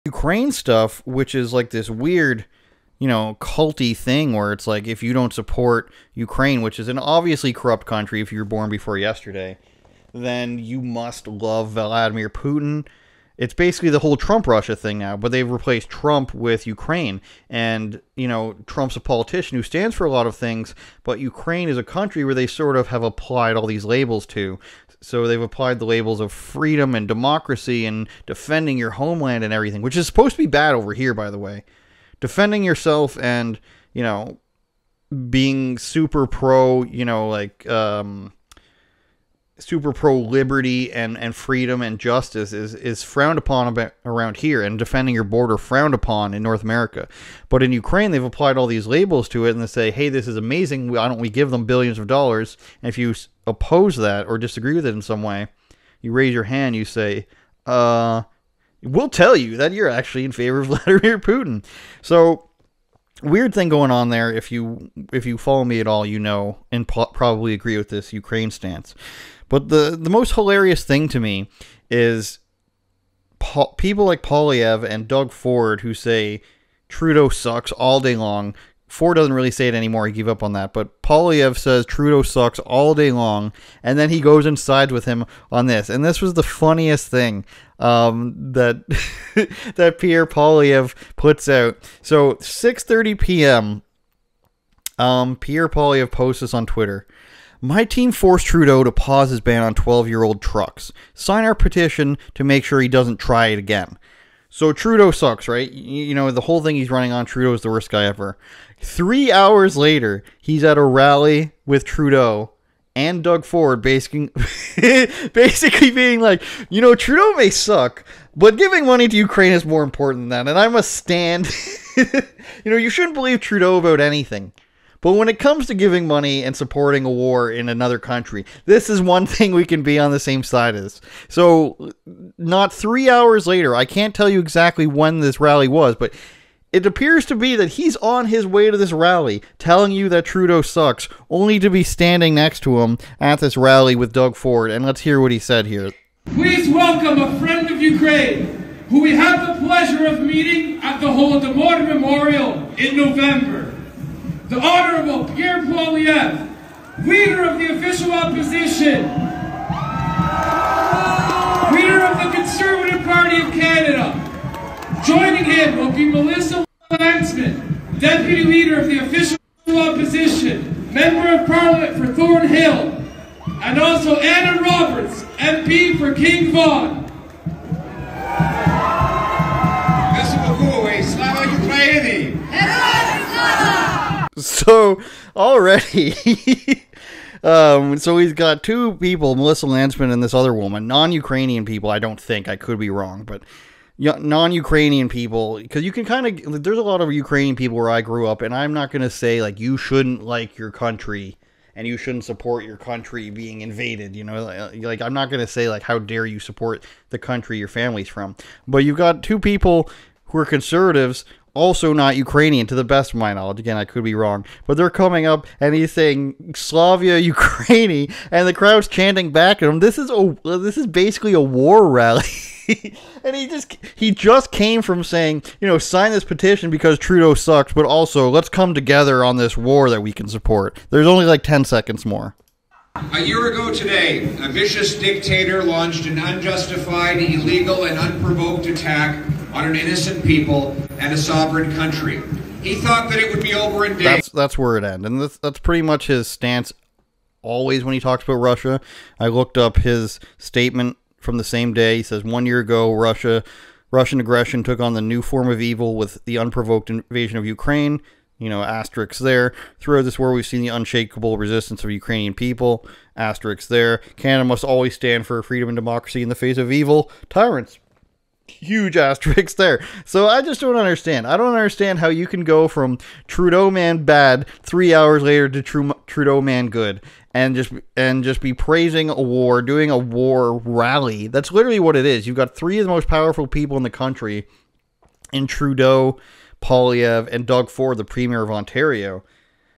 Conservative Party pledges allegiance to Ukraine
Pierre Poilievre criticizes Trudeau on the way to stand with him for Ukraine

ANDREWSAYSTV.COM